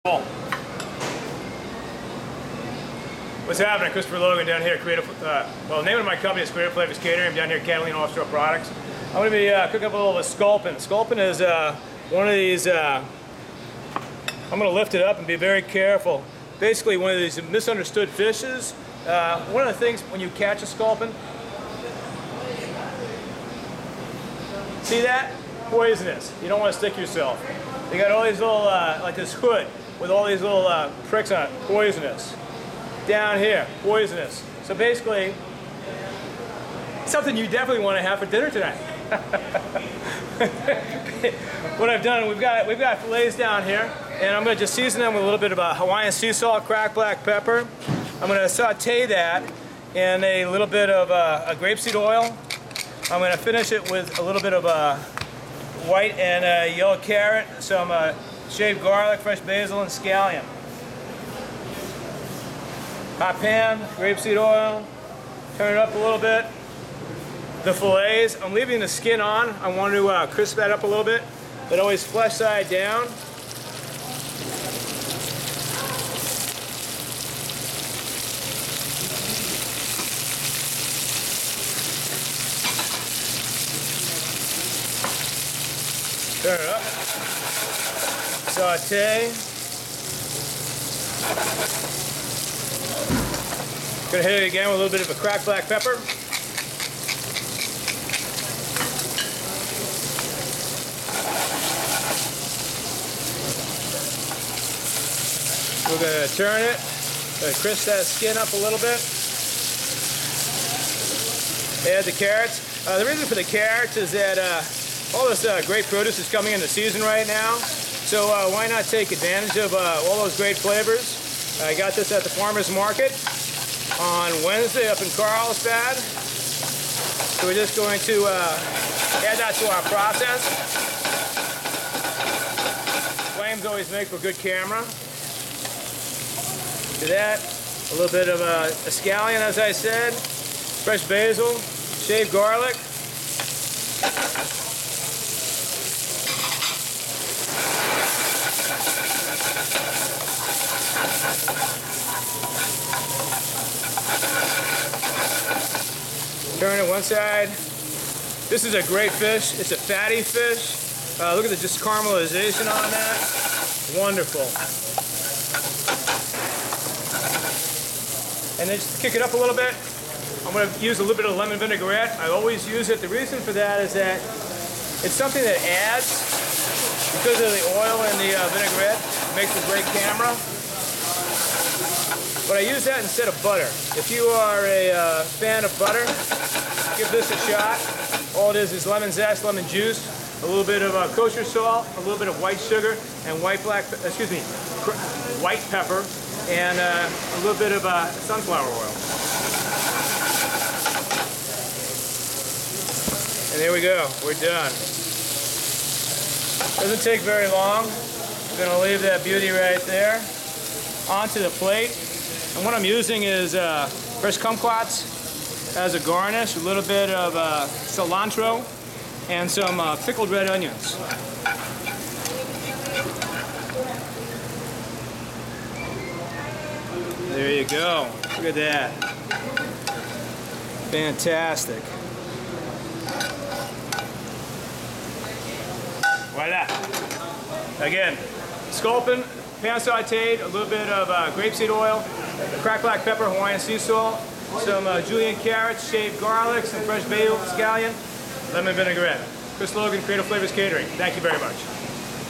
Christopher Logan - Sculpin
Christopher Logan of Creative Flavors Catering prepares some local California Sculpin. Catalina Offshore Products.